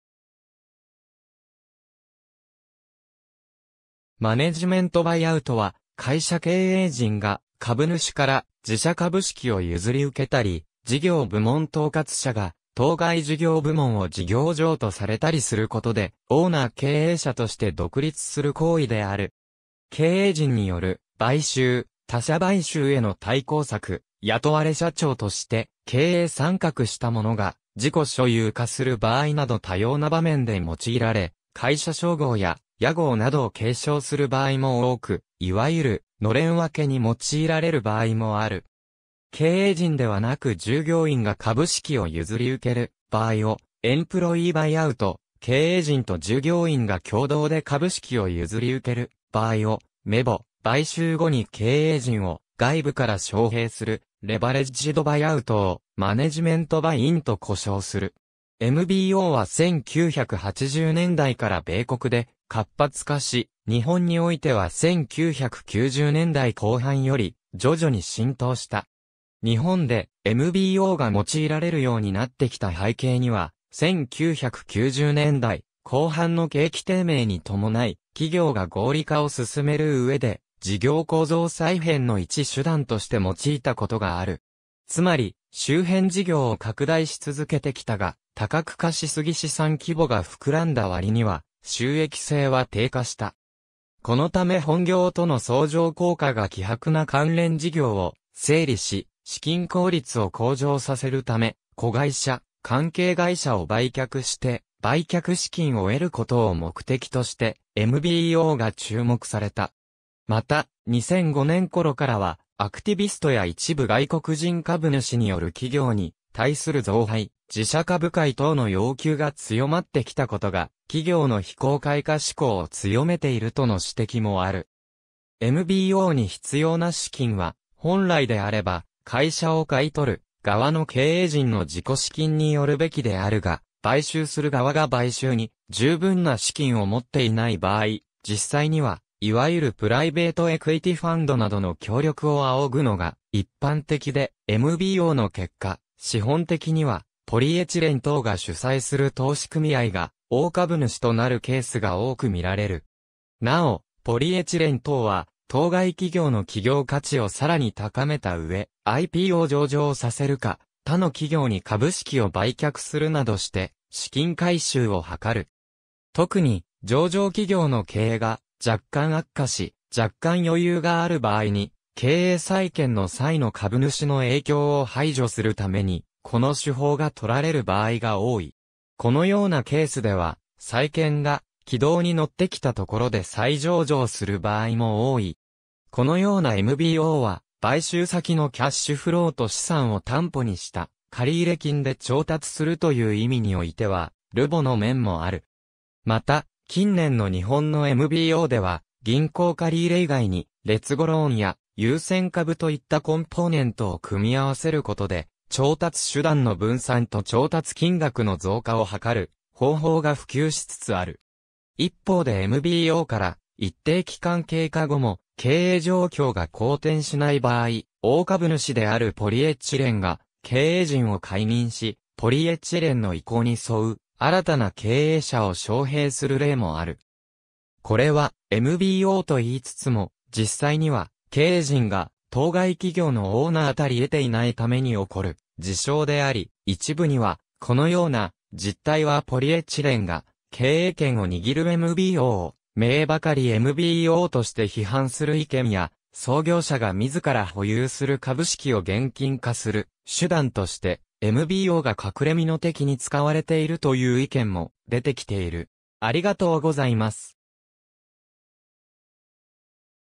マネジメント・バイアウト
マネジメント・バイアウト（MBO、英: management buyout、経営陣買収）は、会社経営陣が株主から自社株式を譲り受けたり、事業部門統括者が当該事業部門を事業譲渡されたりすることで、オーナー経営者として独立する行為である。
経営陣による買収、他者買収への対抗策、「雇われ社長」として経営参画した者が自己所有化する場合など多様な場面で用いられ、会社商号や屋号などを継承する場合も多く、いわゆる「のれん分け」に用いられる場合もある。
経営陣ではなく従業員が株式を譲受る場合をエンプロイー・バイアウト（英: employee buyout）、経営陣と従業員が共同で株式を譲受る場合をMEBO（英: management and employee buyout）、買収後に経営陣を外部から招聘するレバレッジド・バイアウトをマネジメント・バイ・イン（英: banagement buy-in）と呼称する。
MBOは1980年代から米国で活発化し、日本においては1990年代後半より徐々に浸透した。
日本でMBOが用いられるようになってきた背景には、1990年代後半の景気低迷に伴い、企業が合理化を進める上で事業構造再編の一手段として用いたことがある。
つまり、周辺事業を拡大し続けてきたが、多角化し過ぎ資産規模が膨らんだ割には収益性は低下した。
このため本業との相乗効果が希薄な関連事業を整理し、資金効率を向上させるため、子会社・関係会社を売却して売却資金を得ることを目的としてMBOが注目された。
また、2005年頃からはアクティビストや一部外国人株主による企業に対する増配/自社株買い等の要求が強まってきたことが、企業の非公開化志向を強めているとの指摘もある。
MBOに必要な資金は、本来であれば会社を買い取る側の経営陣の自己資金によるべきであるが、買収する側（経営陣）が買収に十分な資金を持っていない場合、実際にはいわゆるプライベート・エクイティ・ファンド（PE）などの協力を仰ぐのが一般的で、MBOの結果、資本的にはPE等が主宰する投資組合が大株主となるケースが多く見られる。
なお、PE等は当該企業の企業価値をさらに高めた上、IPO（株式（再）上場）させるか、他の企業に株式を売却するなどして、資金回収を図る。
特に、上場企業の経営が若干悪化し、若干余裕がある場合に、経営再建の際の株主の影響を排除するために、この手法がとられる場合が多い。
このようなケースでは、再建が軌道に乗ってきたところで再上場する場合も多い。
このようなMBOは、買収先のキャッシュフローと資産を担保にした借入金で調達するという意味においては、LBO（レバレッジド・バイアウト）の面もある。
また、近年の日本のMBOでは銀行借入以外に劣後ローンや優先株といったコンポーネント（いわゆる「メザニン」）を組み合わせることで、調達手段の分散と調達金額の増加を図る方法が普及しつつある。
一方でMBOから一定期間経過後も経営状況が好転しない場合、大株主であるPEが経営陣を解任し、PEの意向に沿う新たな経営者を招聘する例も有る（すかいらーくが代表例）。
これはMBOと言いつつも、実際には経営陣が当該企業のオーナーたり得ていないために起こる事象であり、一部にはこのような「実態はPEが経営権を握るMBO」を「名ばかりMBO」として批判する意見 や、「創業者が自ら保有する株式を現金化する手段としてMBOが隠れ蓑的に使われている」という意見 も出てきている。